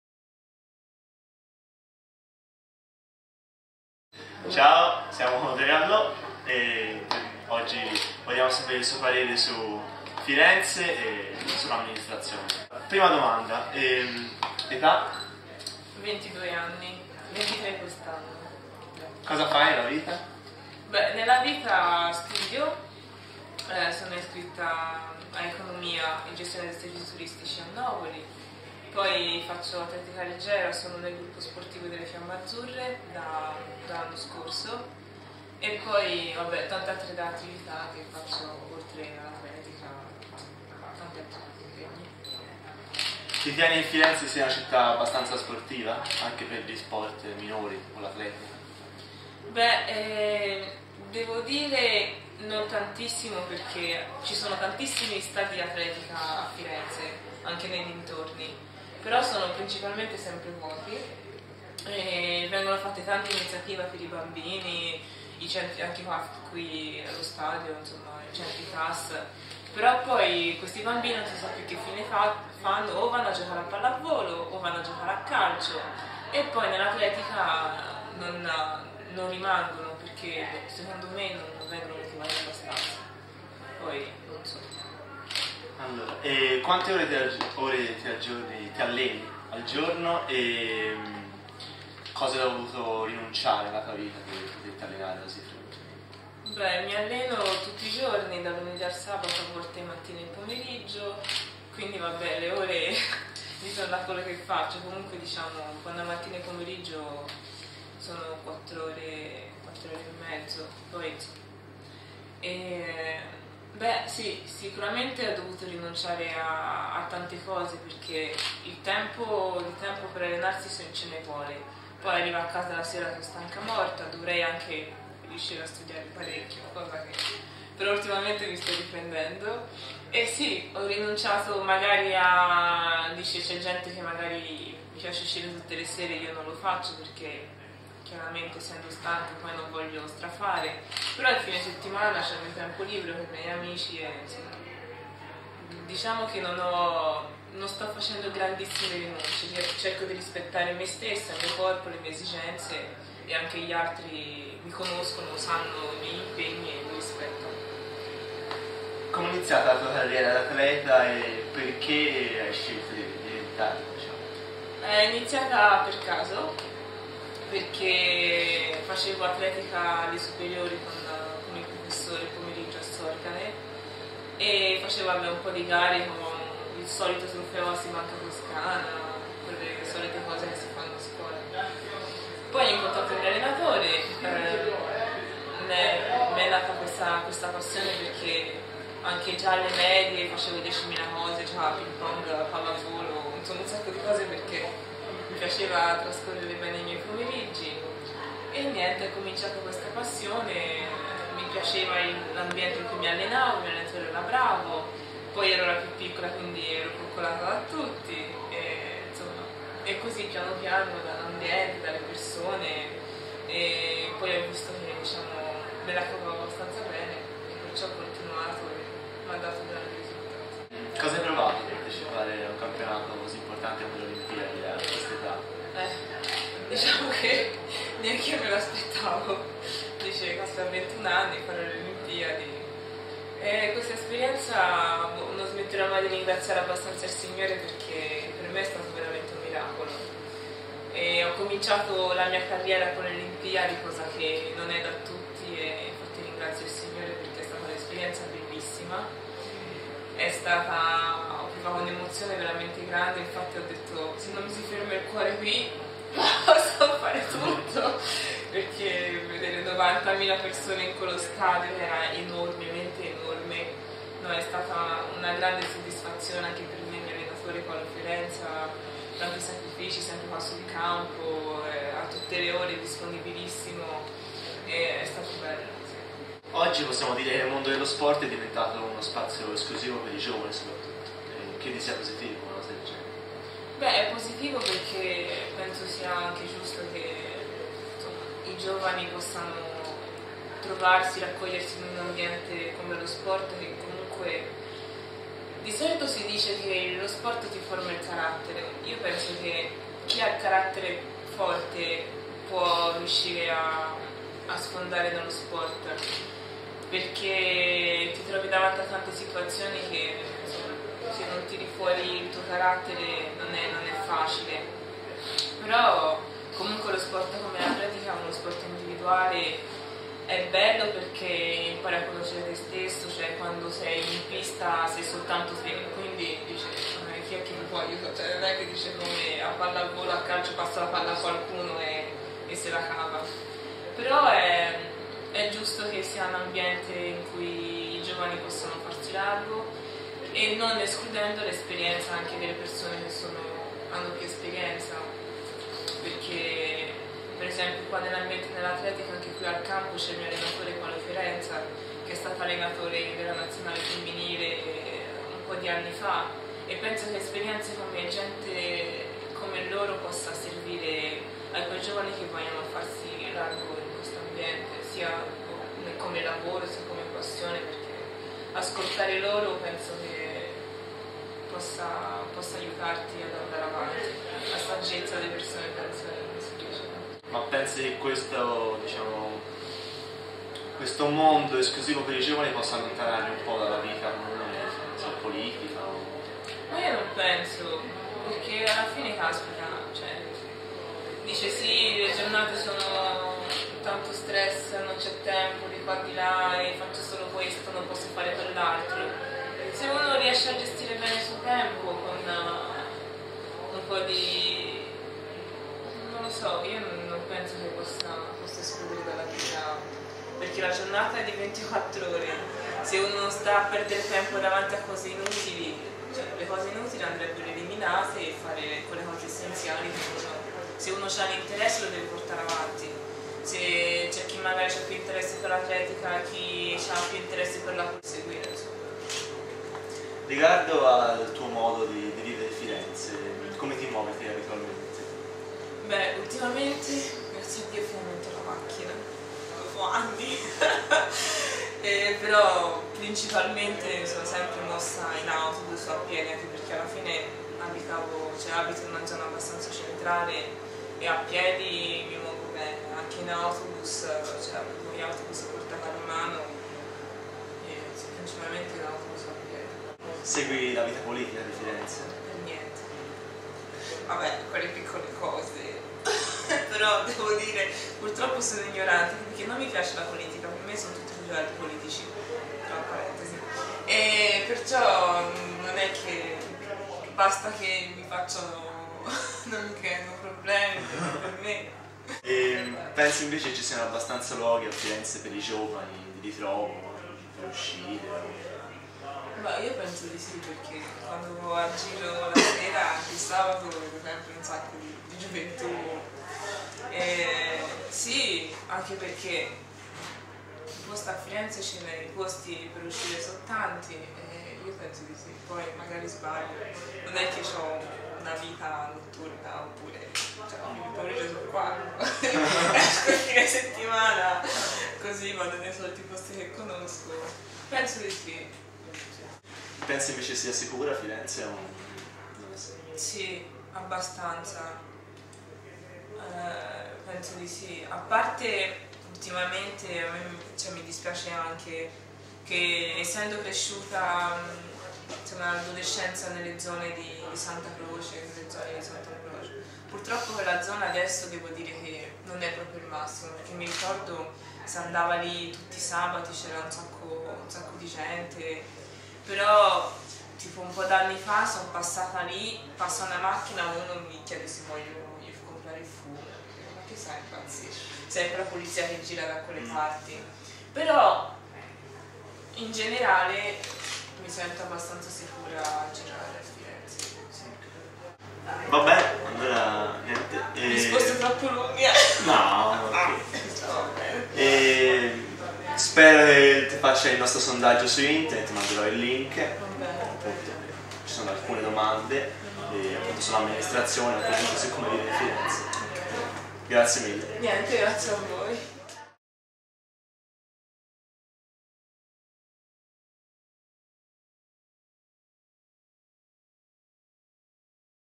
Intervista a Audrey Alloh
Gruppo Ripplemarks in azione.
Intervista all'atleta Audrey Alloh, in seguito al sondaggio lanciato da Ripplemarks su

Francesco fa le domande, Alice riprende, Bernardo fotografa.
Montaggio: Riccardo.